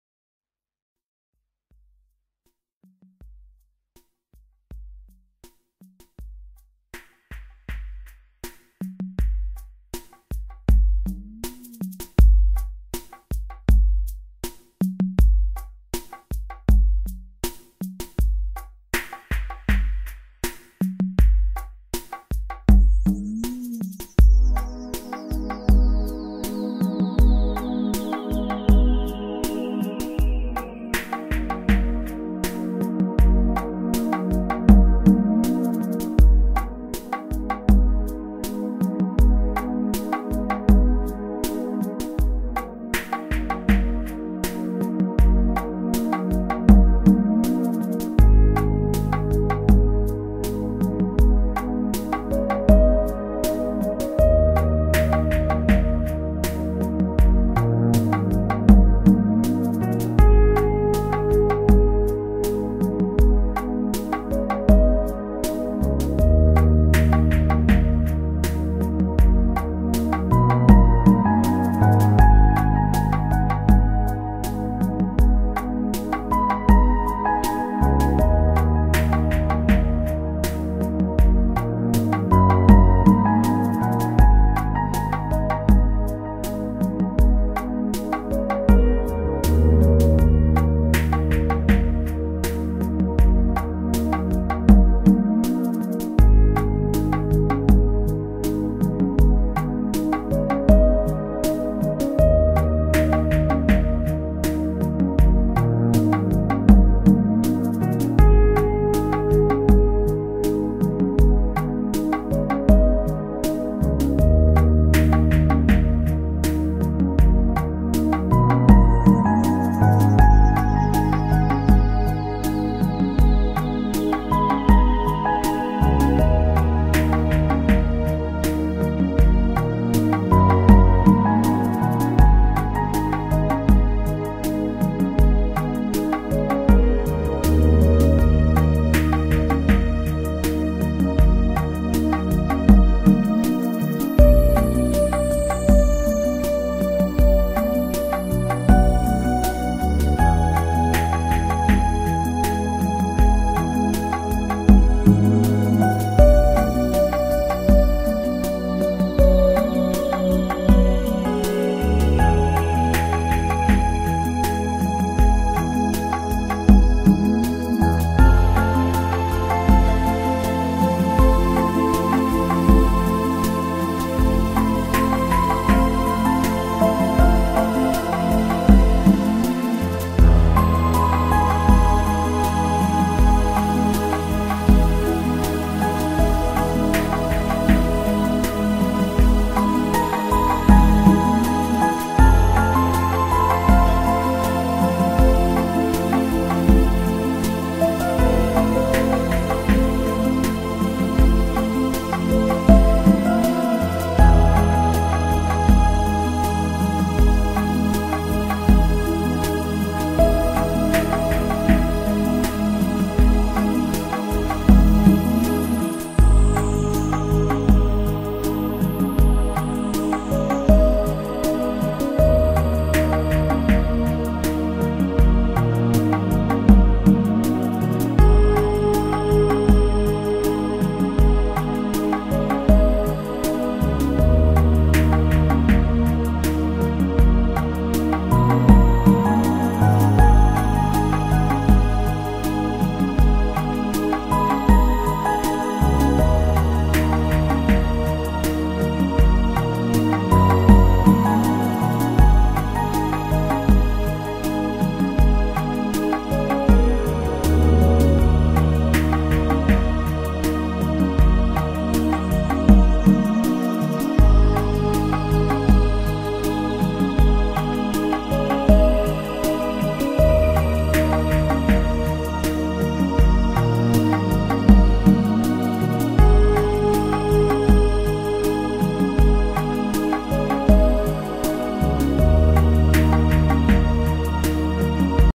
I M A G I N E ~ The World of Fractal Art, Vol 1
A short video showing some of my own fractal art.  All images were created with Fractal Explorer or Ultra Fractal programs, with post-processing in Photoshop.  Music performed by David Schombert.  You can find his album and listen/download for Free